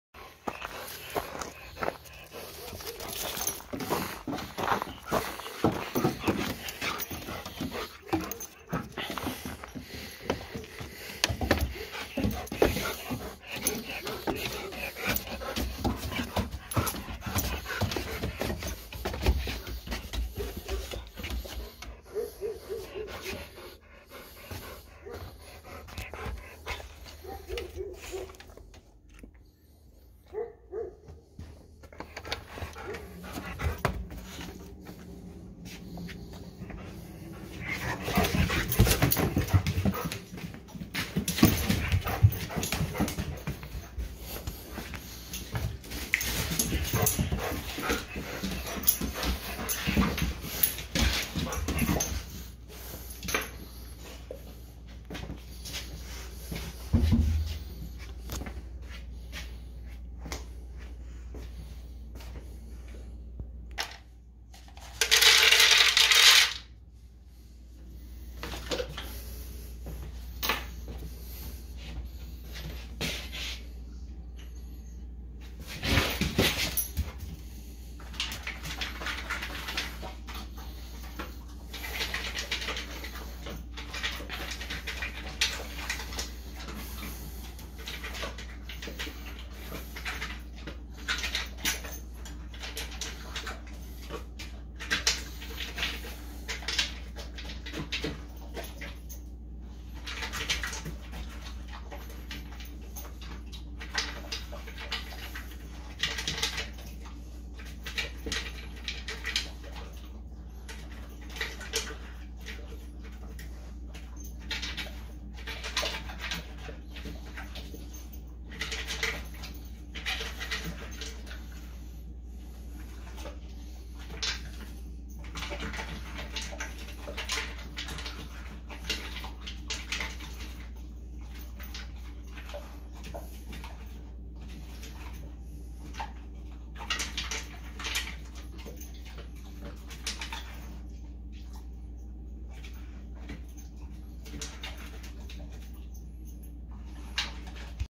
We getting food? I bet we're getting food?
Hazel is always super excited for food.